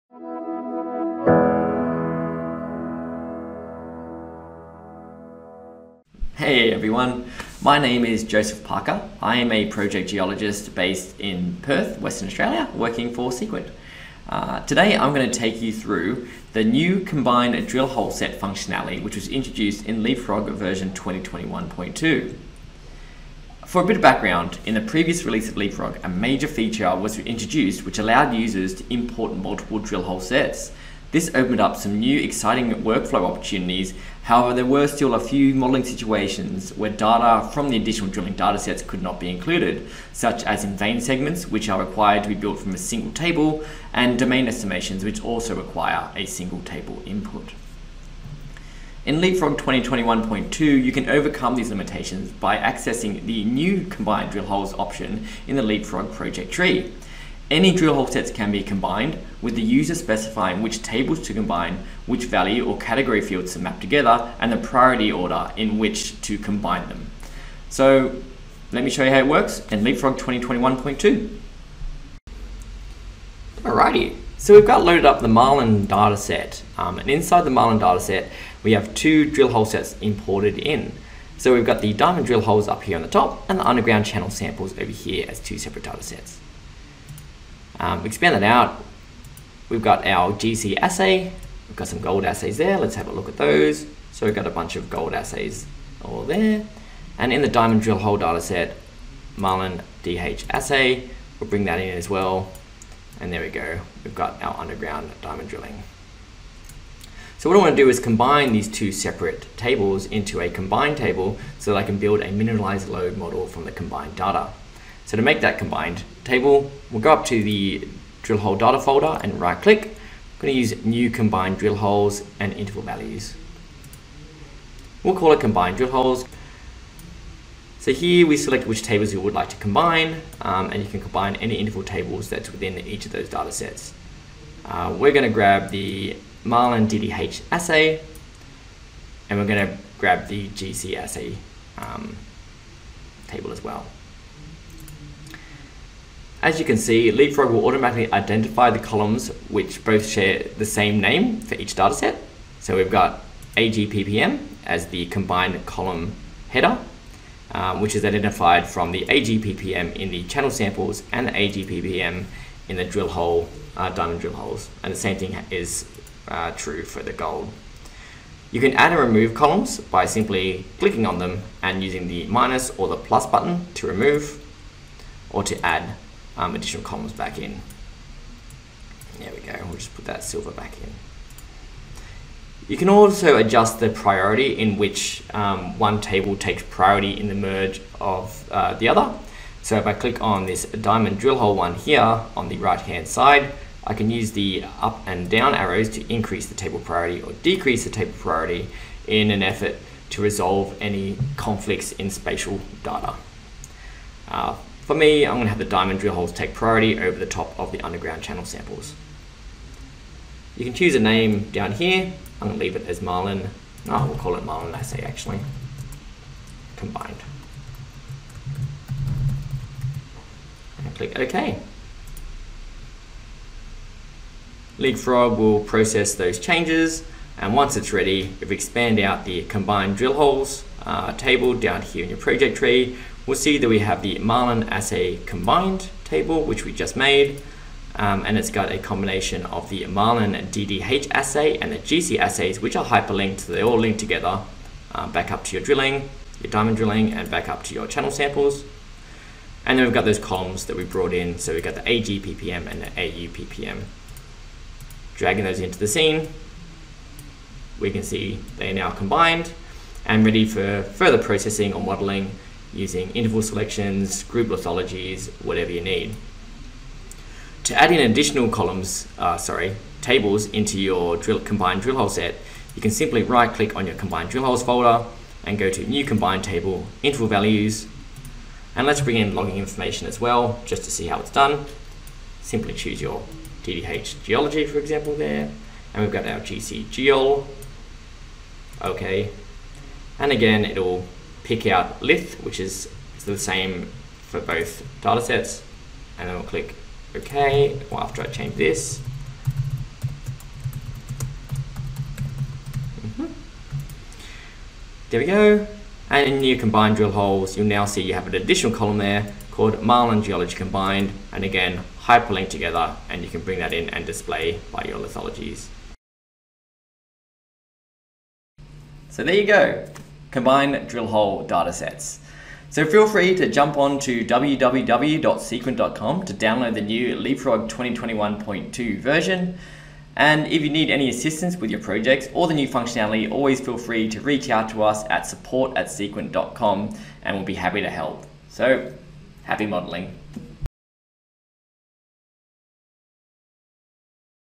Use Cases for Merged Drillhole Sets in Leapfrog Geo 2021.2
The latest release of Leapfrog Geo includes a multitude of new features, enhancements and upgrades.

In this video, we’ll explore use cases and improvements to merged drillhole sets. Joseph Parker will share use cases and insights into the latest version of Leapfrog Geo 2021.2, and the enhancements we’ve made to merged drillhole sets.